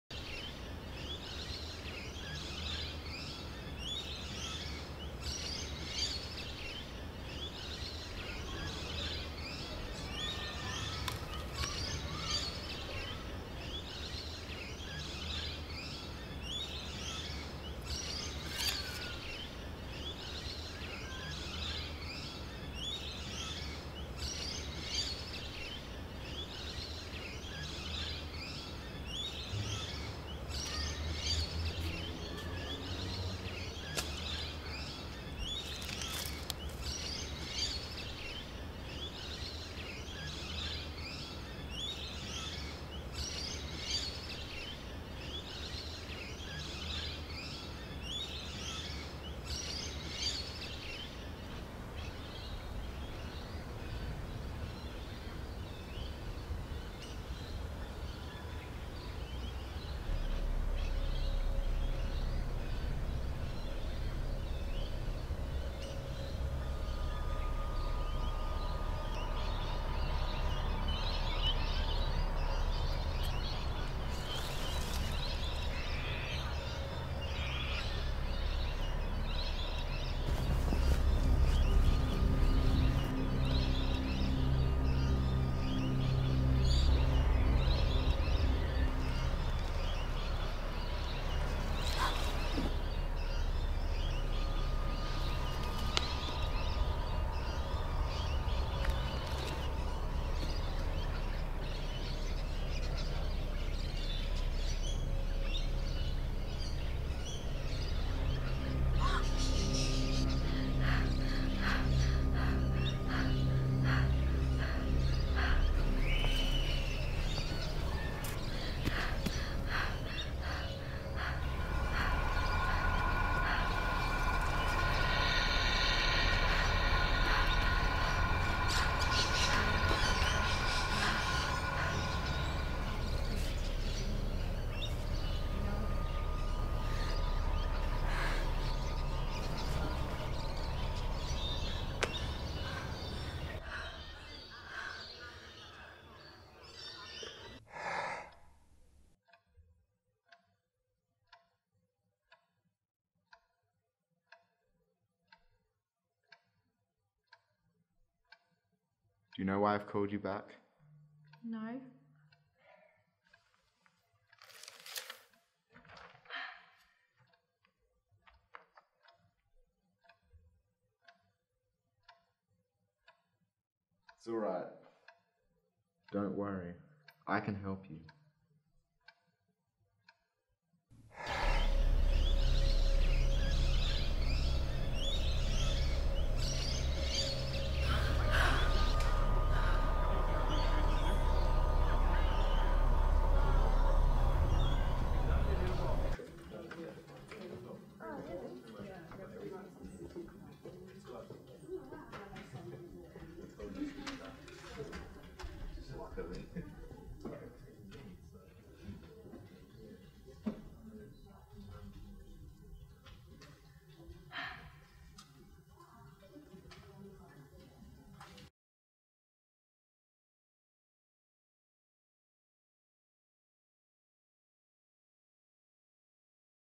Breathe
A short film for uni. Had to be a drama with obstacles and a goal. Our idea was vague and rushed so I don't think it turned out as well as it could've. Maybe better sound design could've saved it to make it truly creepy.